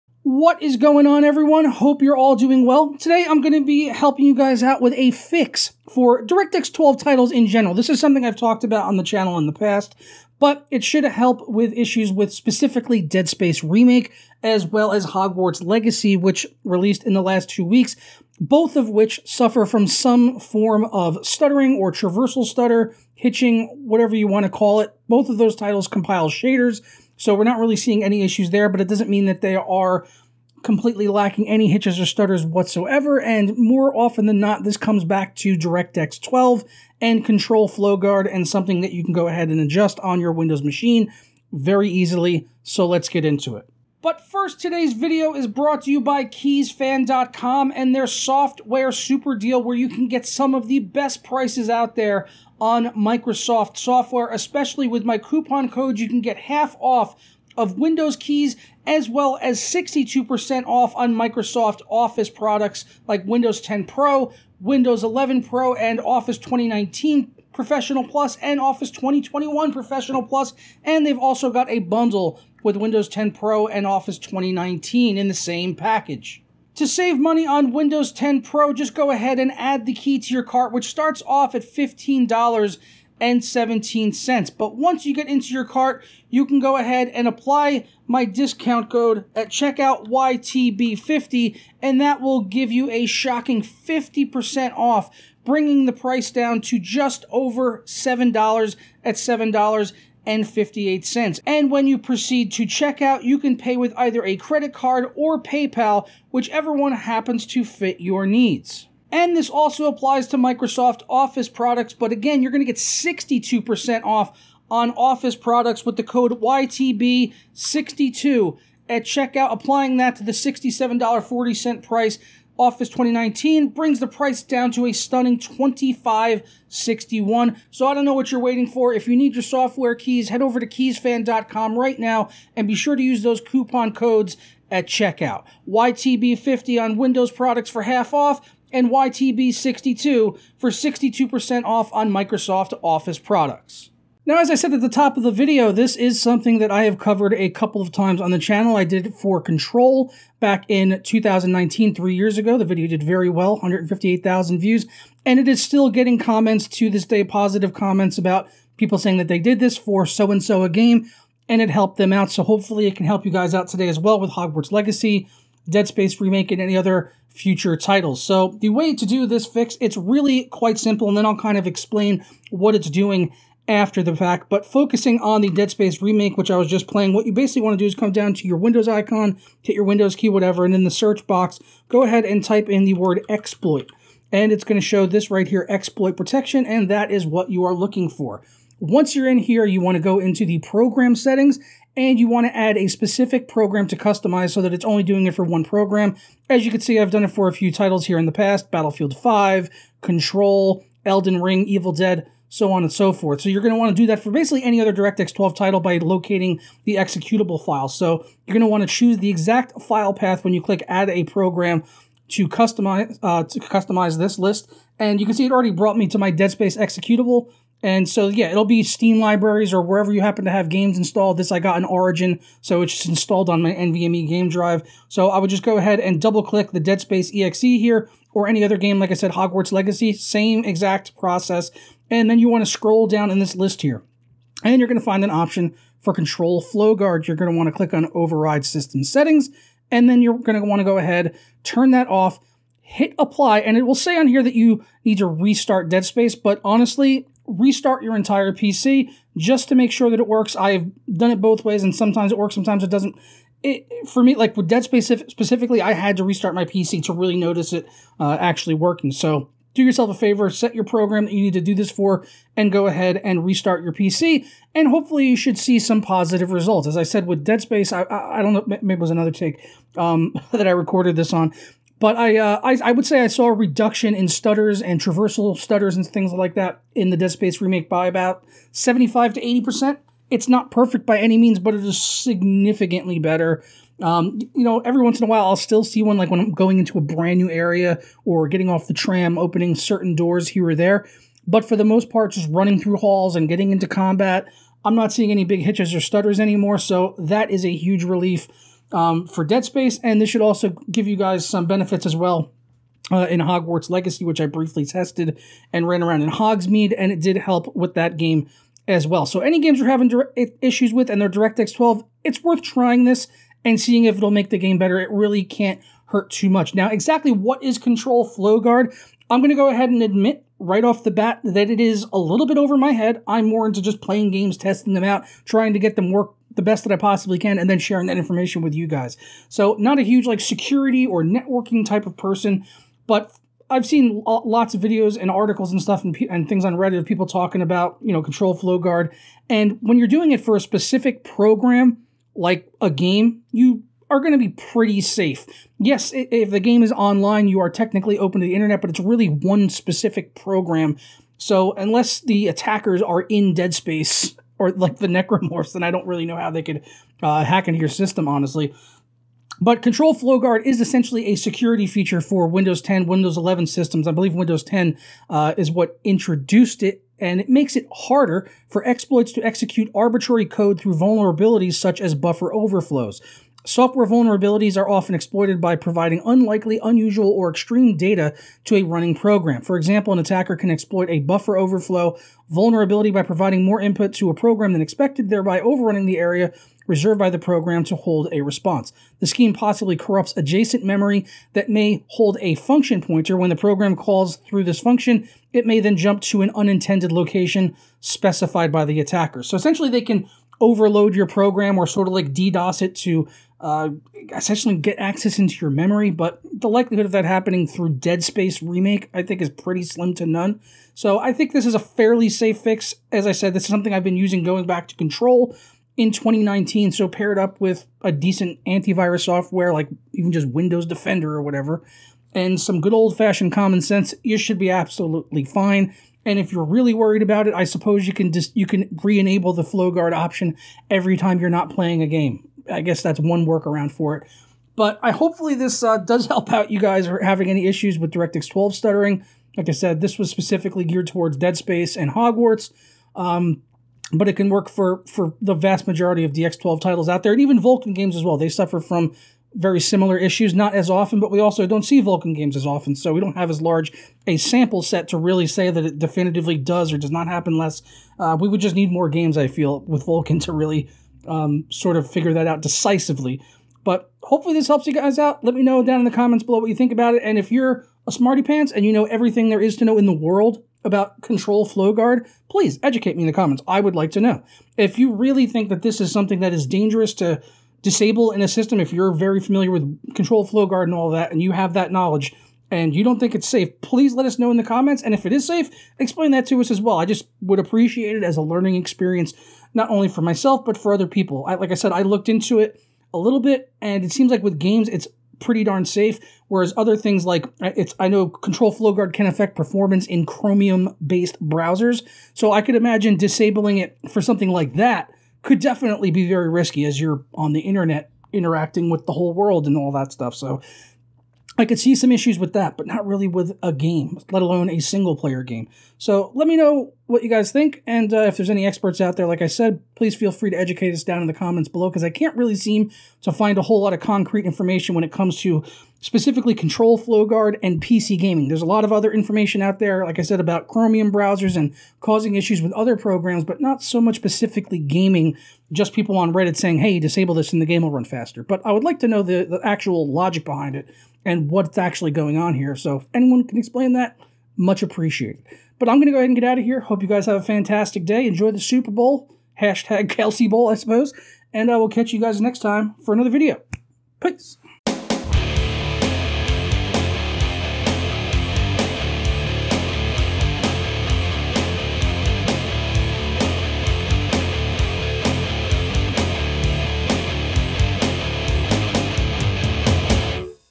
How to Fix DirectX 12 Stutter in 2023 | Hogwarts Legacy, Dead Space & more
Buy Windows and Office cheaply and legally.

Timestamps
0:00 Intro
0:43 Keysfan Ad Spot
2:50 How to Guide
5:30 What is Control Flow Guard